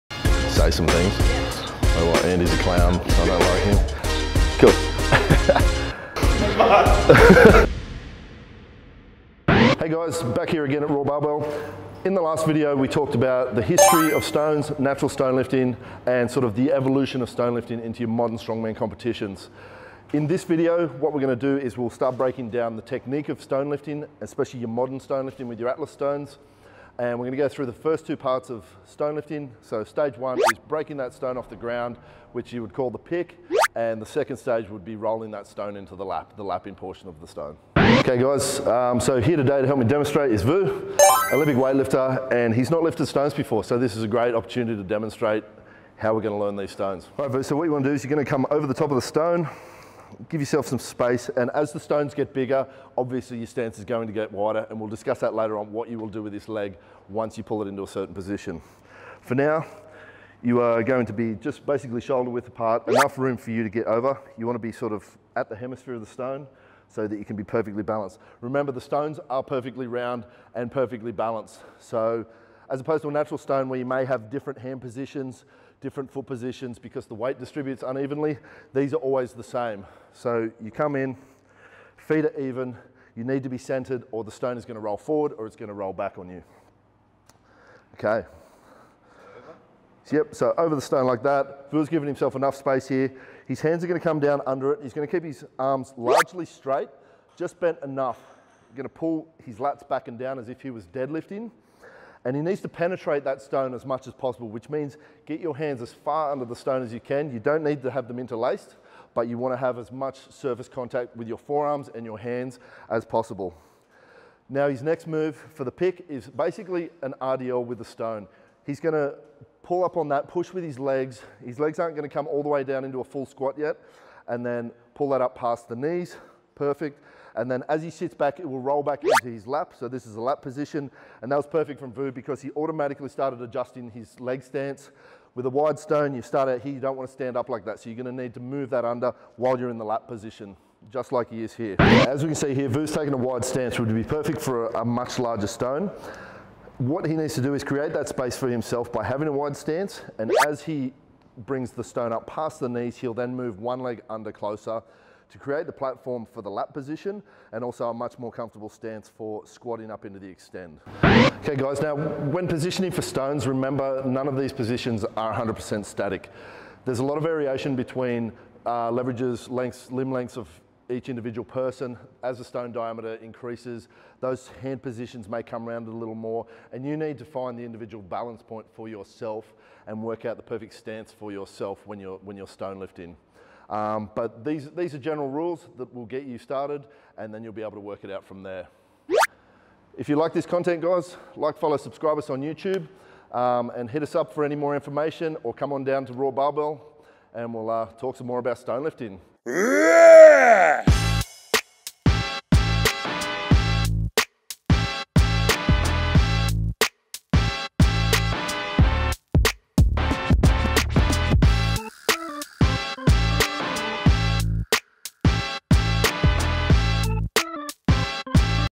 How To Lift an Atlas Stone : The Pick and Lap (Strongman Stones Tutorial)
Professional Strongman Luke Reynolds teaches The Snatch Sensei, Vu how to pick up and lap an Atlas Stone. One of Strongman’s most popular events, learning how to lift stones is a must if you are interested in Strongman Training.

Luke demonstrates and points out common mistakes and flaws in stone lifting technique as well how to correctly lift a stone.

Try these in your next strongman workout! Remember proper technique will help stave of injuries and make your lifting better!